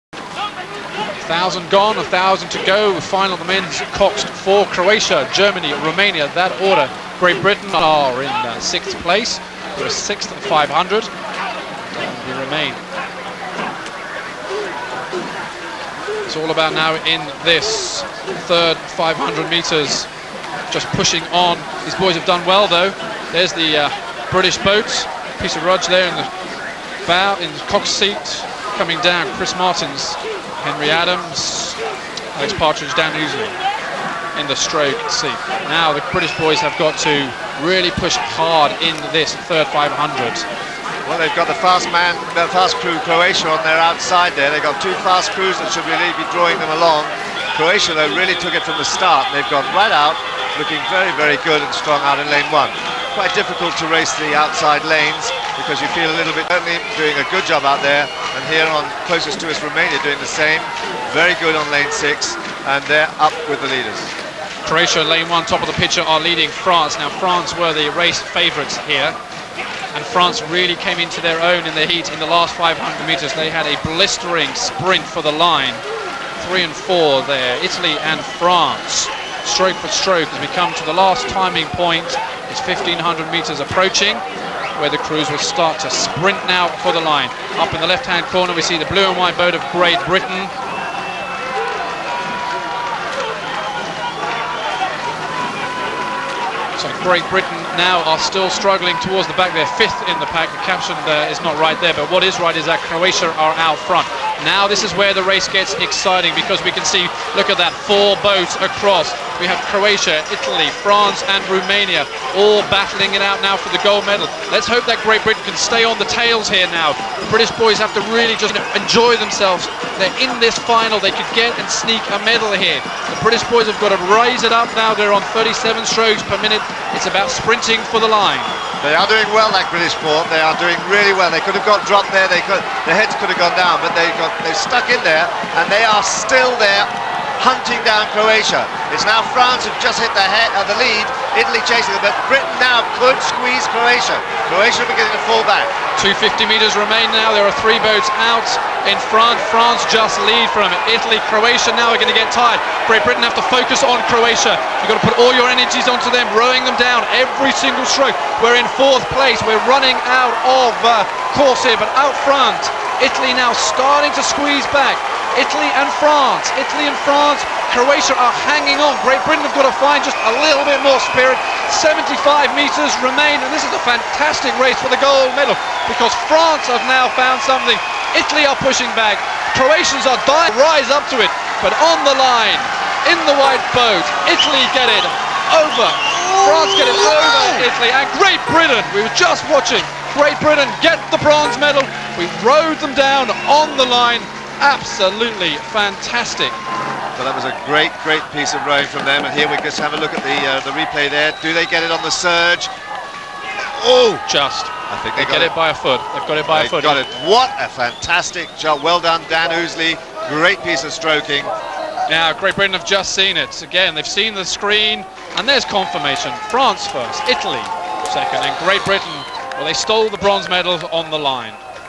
Lucerne M4+ 2001
The final of the coxed fours 4+ rowing event in Lucerne, Switzerland. Look out for the rather tight finish that the British crew have on the line. What a photo finish